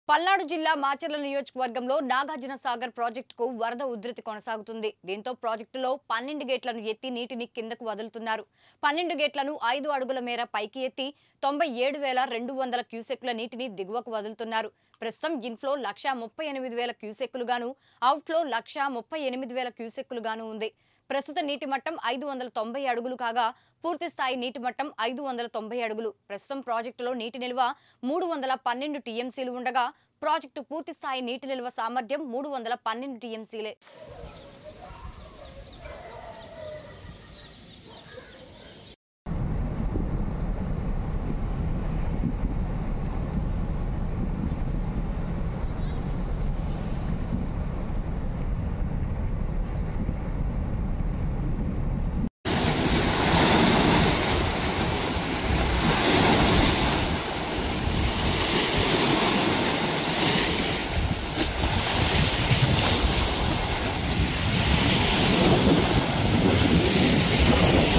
నాగార్జున సాగర్ || Nagarjuna sagar project 12 gates lifted due to heavy flooding || Mega9tv News
పల్నాడు జిల్లా మాచర్ల నియోజకవర్గంలో నాగార్జున సాగర్ ప్రాజెక్టుకు వరద ఉధృతి కొనసాగుతోంది. దీంతో ప్రాజెక్టులో  12 గేట్లను ఎత్తి నీటిని కిందికి వదులుతున్నారు. 12 గేట్లను ఐదు అడుగుల మేర పైకి ఎత్తి  97,200 క్యూసెక్కుల  నీటిని దిగువకు వదులుతున్నారు. ప్రస్తుతం ఇన్ ఫ్లో లక్షా 38 వేల క్యూసెక్కులుగానూ, ఔట్ ఫ్లో  లక్షా 38 వేల క్యూసెక్కులుగానూ ఉంది.
ప్రస్తుత నీటి మట్టం 590 అడుగులు కాగా... పూర్తి స్థాయి నీటి మట్టం 590 అడుగులు. ప్రస్తుతం ప్రాజెక్టు లో నీటి నిల్వ  312 టీఎంసీలు ఉండగా.. ప్రాజెక్టు పూర్తి స్థాయి నీటి నిల్వ సామర్థ్యం 312 టీఎంసీలే.